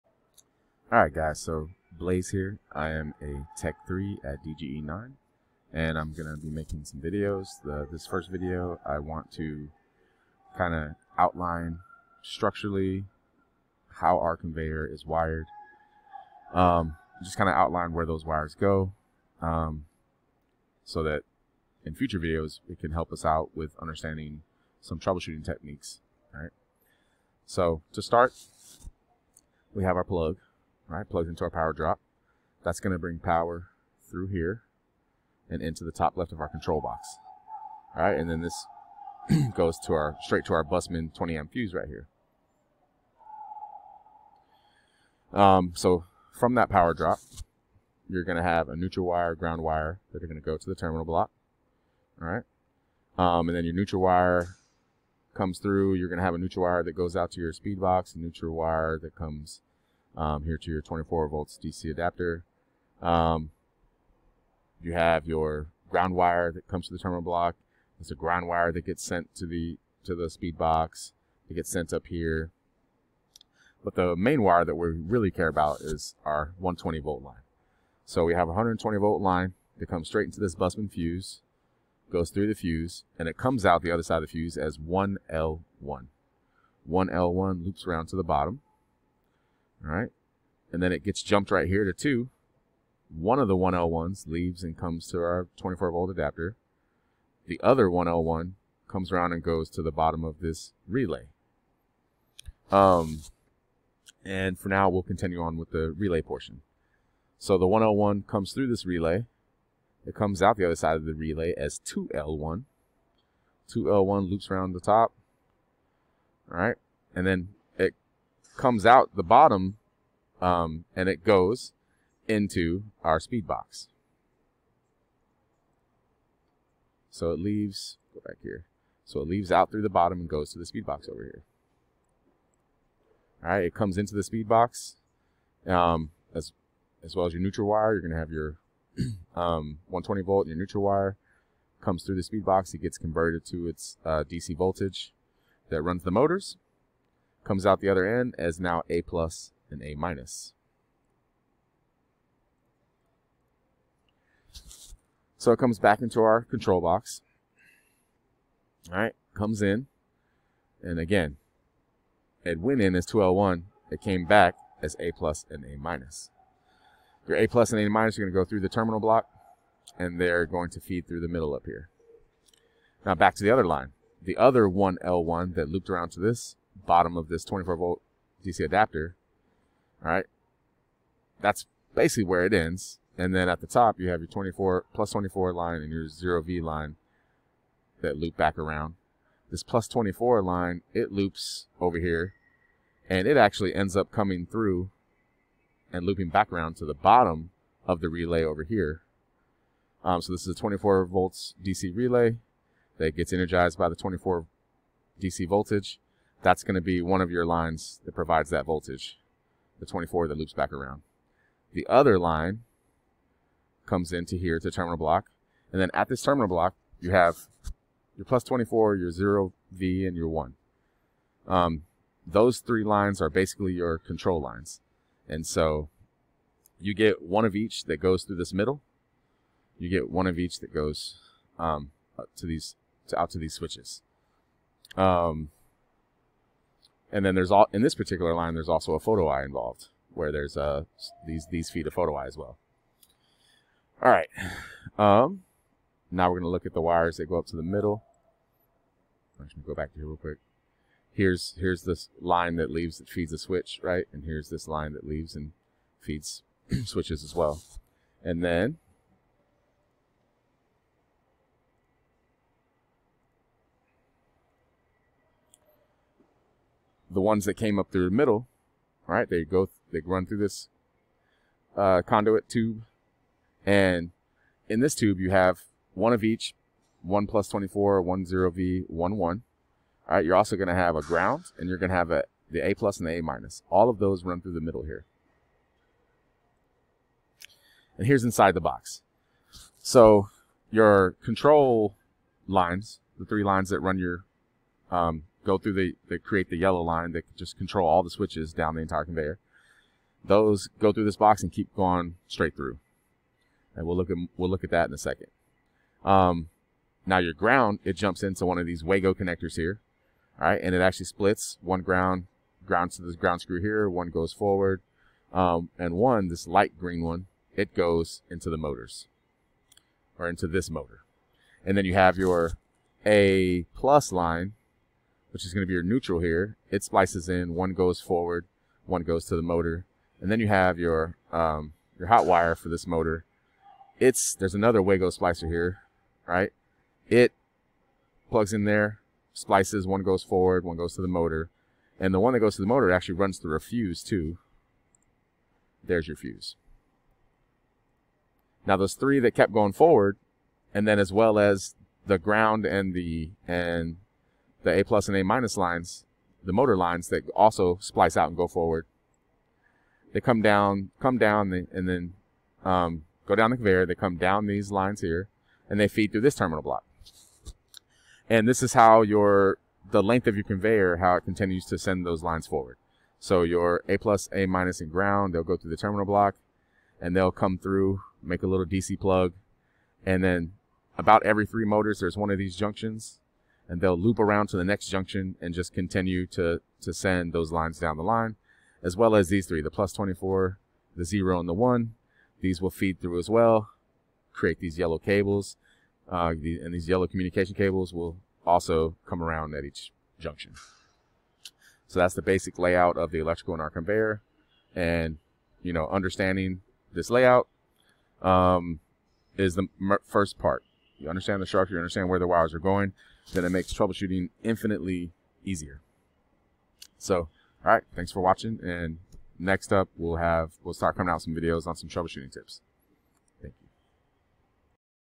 Electrical structure of conveyor at DGE9
Basic electrical layout of conveyor system at DGE9 (Gen 1 Roach Site). This video will serve to establish a baseline understanding of the wiring structure from a hands on perspective. That understanding provides invaluable information toward effective troubleshooting when required.